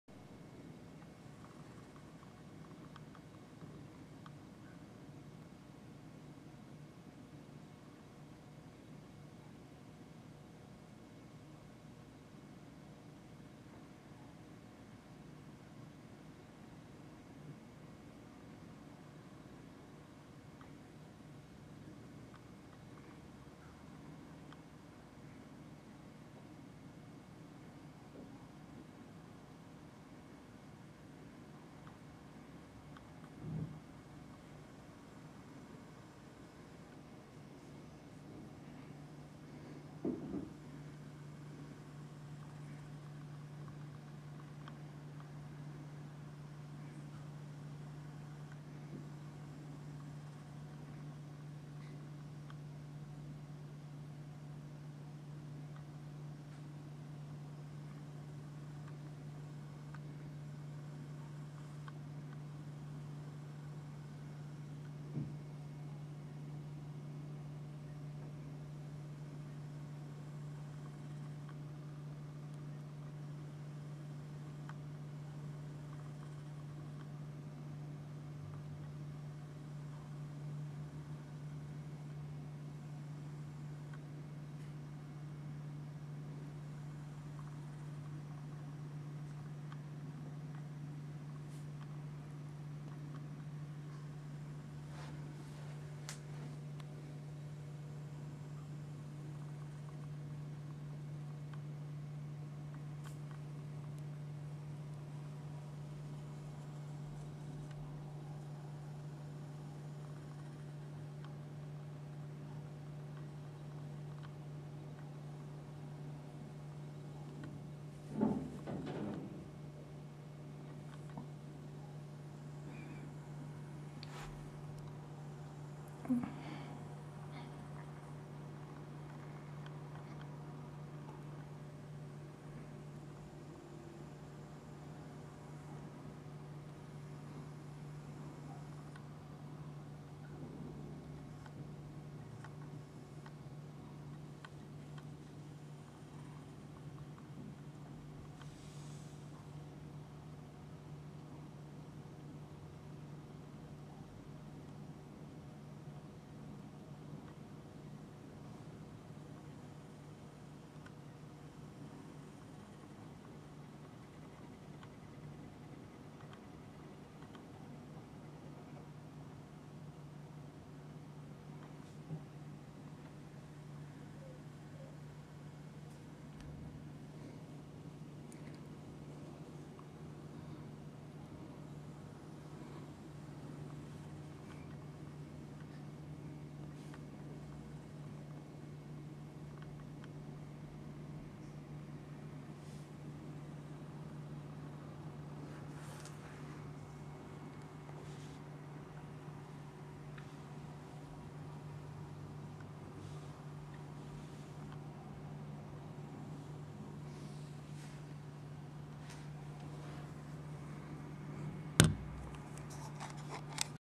Vancouver Winter Olympic 2010- Calgary Tower Celebrating by lighting its Torch
Torch for Winter Olympic  2010